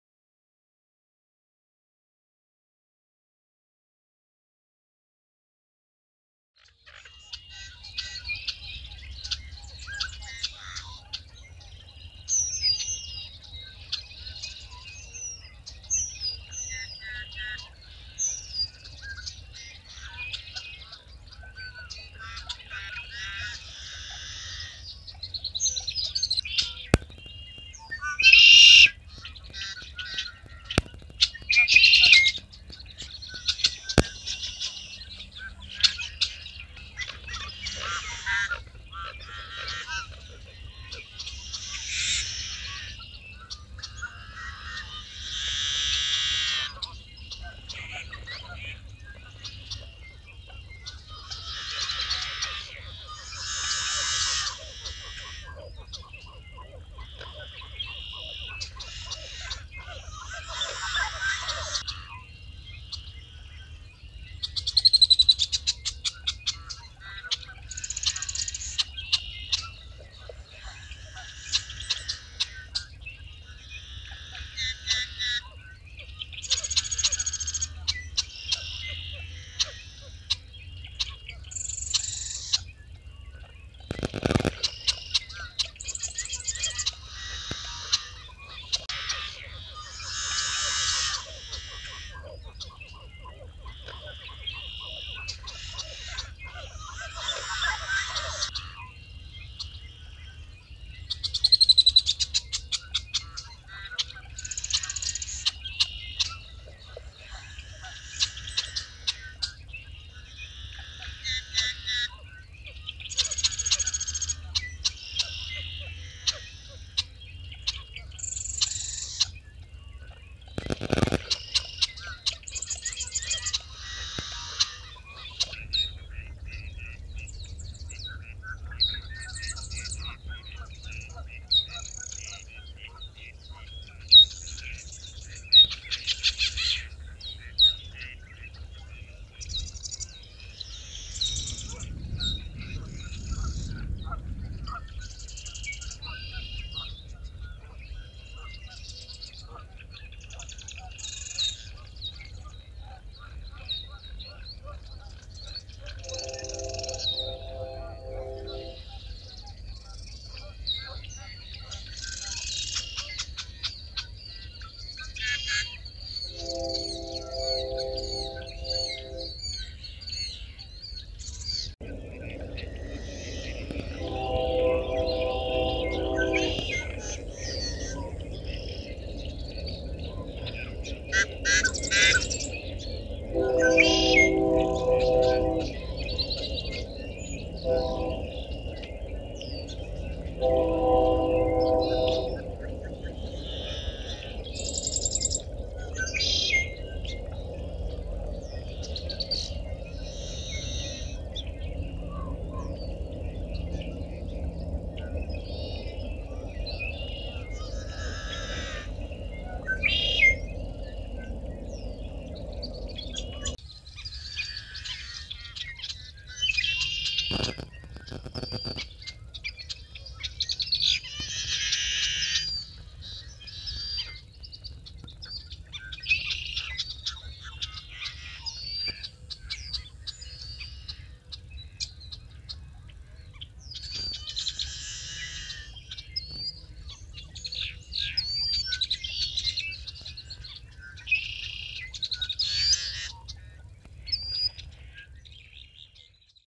The sounds of a marsh awakening
A homemade parabolic dish and remote microphone used to capture the sounds of a marsh a dawn.